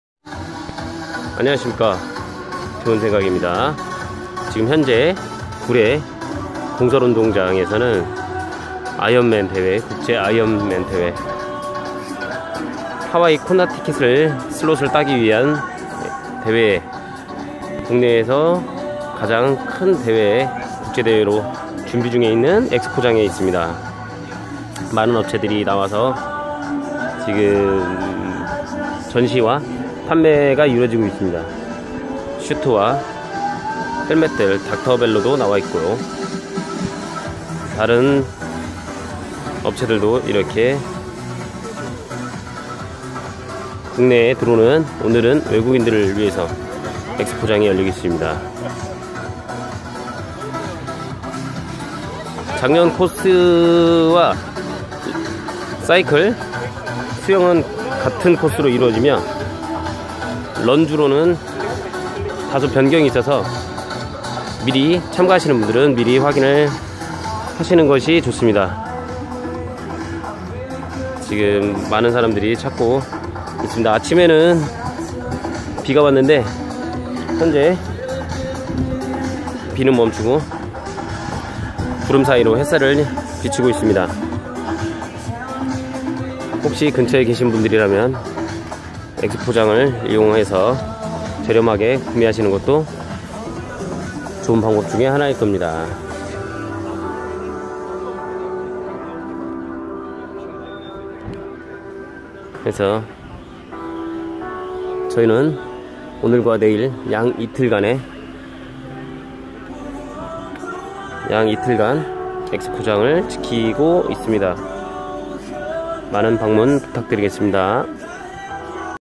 2018 구례아이언맨대회 엑스포장
구례아이언맨대회 엑스포장
하와이 코나대회 슬롯을 따기 위해
한 해 열심히 한 철인 분들
가슴이 설레이고 있는 장소 입니다